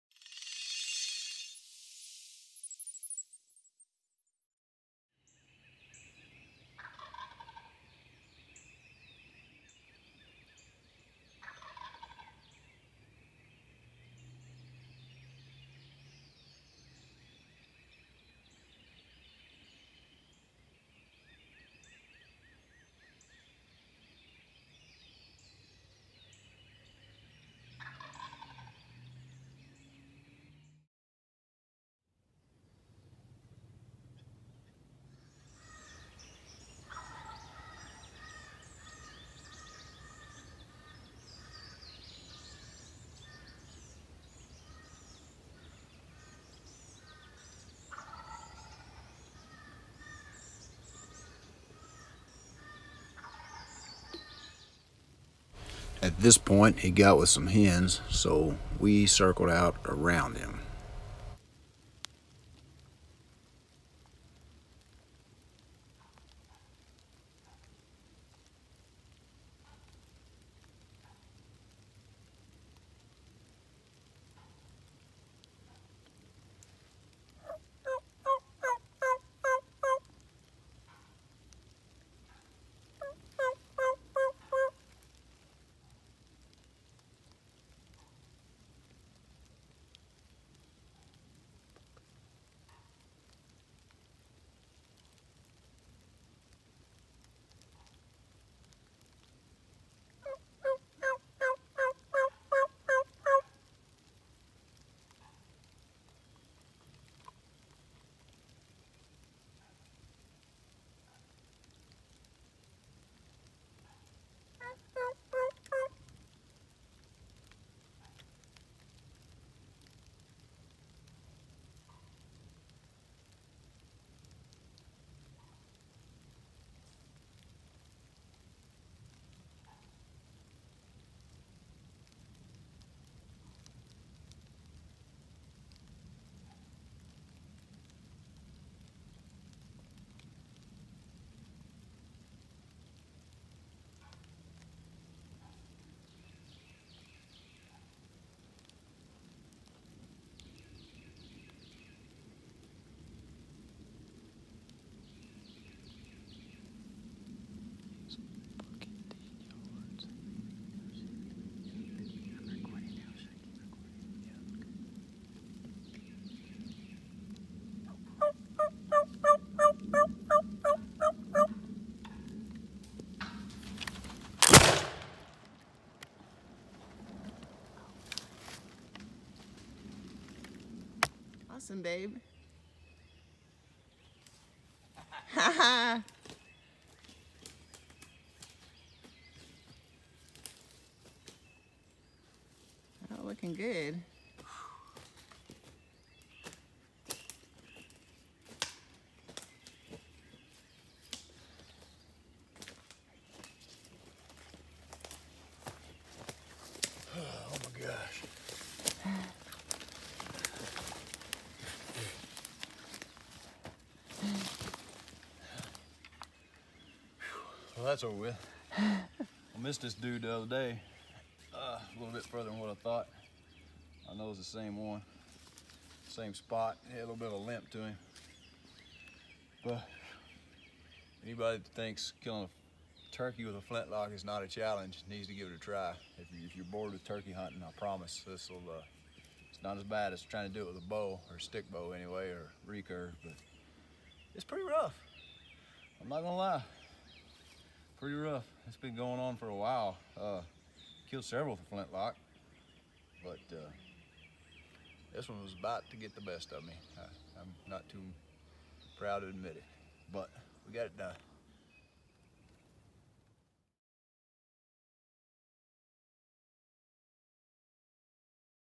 Turkey Hunting Spring Gobblers with a Flintlock
Taking a traditional.75 smoothbore flintlock into the spring woods in search of a gobbler. Black powder traditional equipment and mouth calling adding up to success!

Facebook Ramshackle Homestead with Keith and Jen